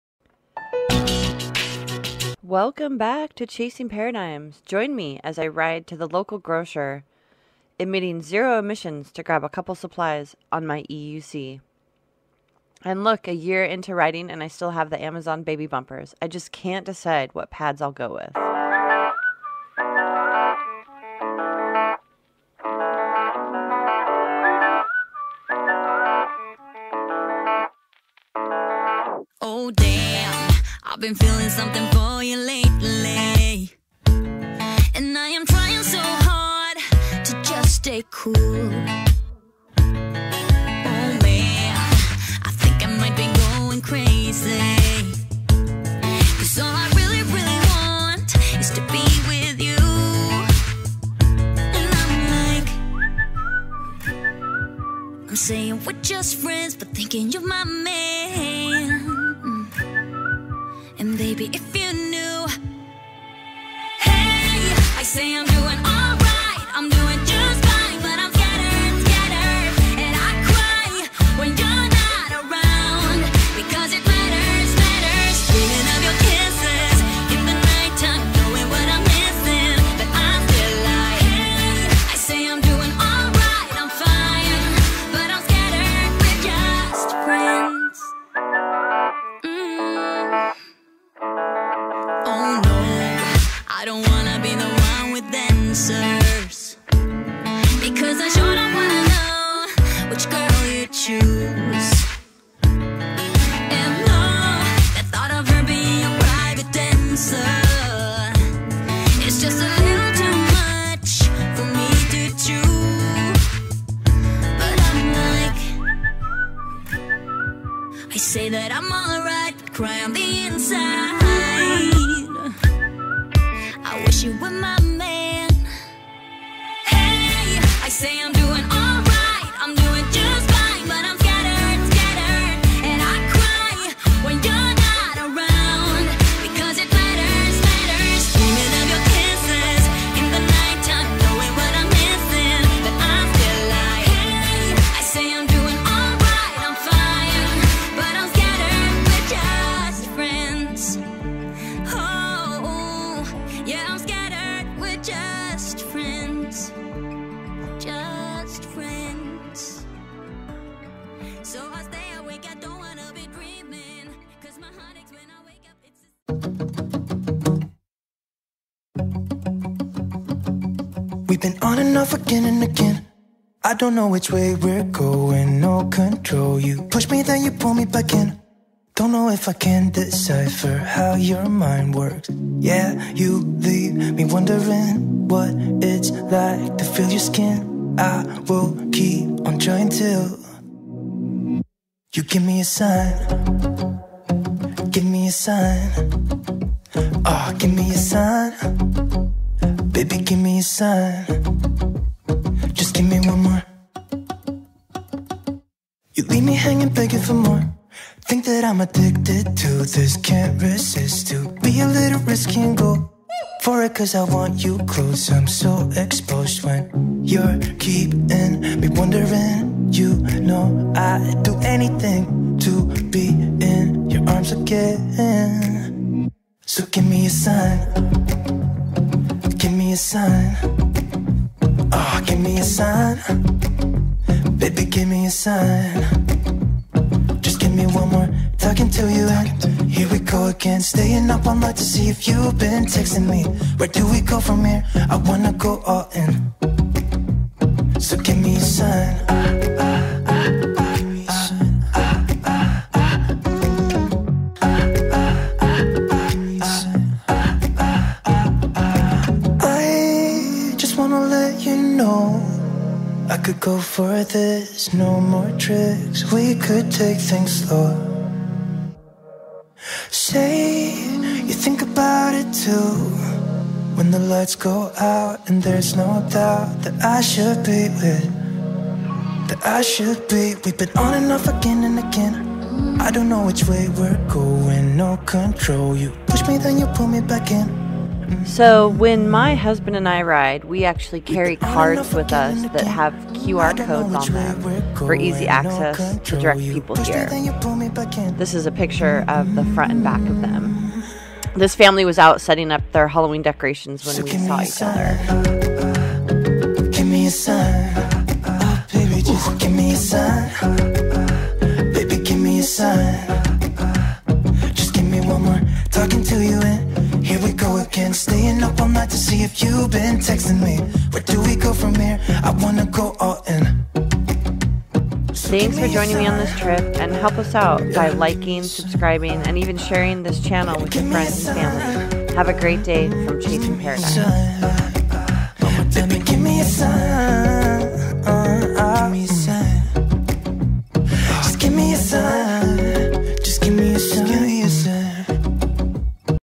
No Crashes 😁, EUC trip, Zero Emissions to Grocery Store on Electric Unicycle
Come with me to the grocery store - its just a quick trip, I promise. 😉